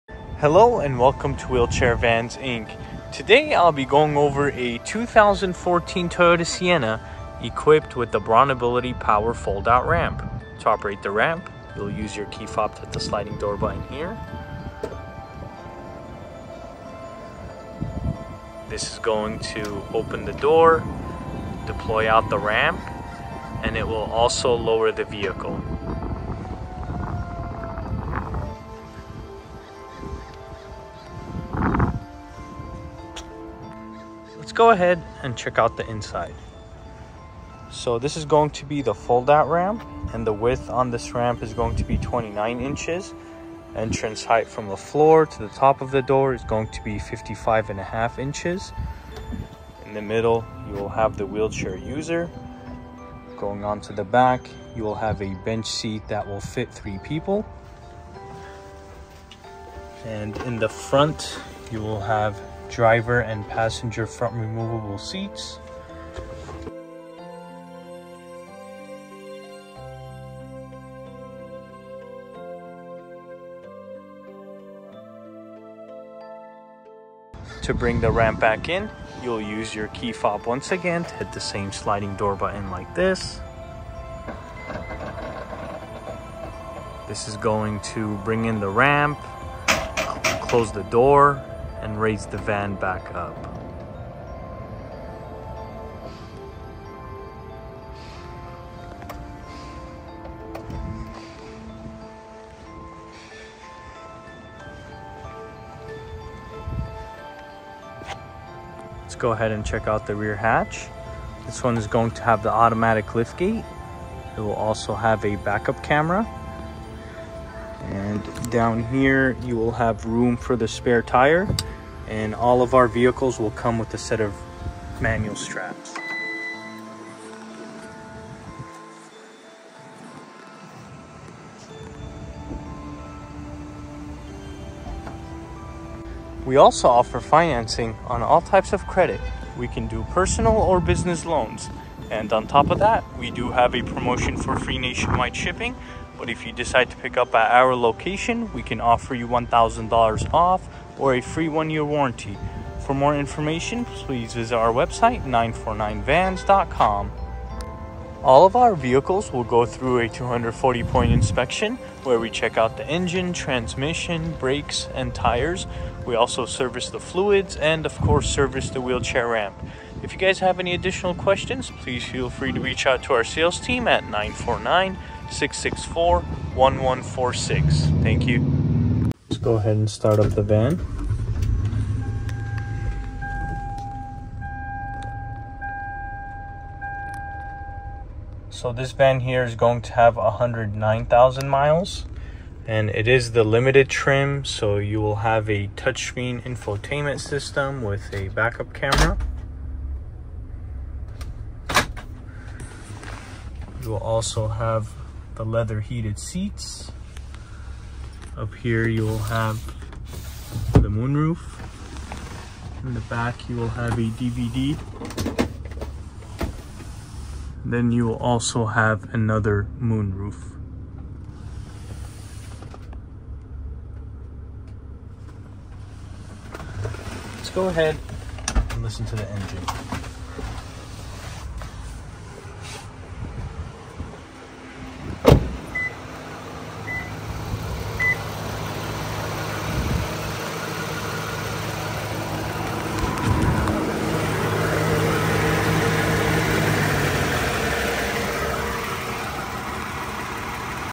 For Sale 2014 Toyota Sienna BraunAbility Power ONE TOUCH Fold Out Ramp Side Loading Wheelchair Van
Sale priced $31,995 with Free Nationwide Shipping For Sale 2014 Toyota Sienna Limited BraunAbility Power ONE TOUCH Fold Out Ramp Side Loading Wheelchair Van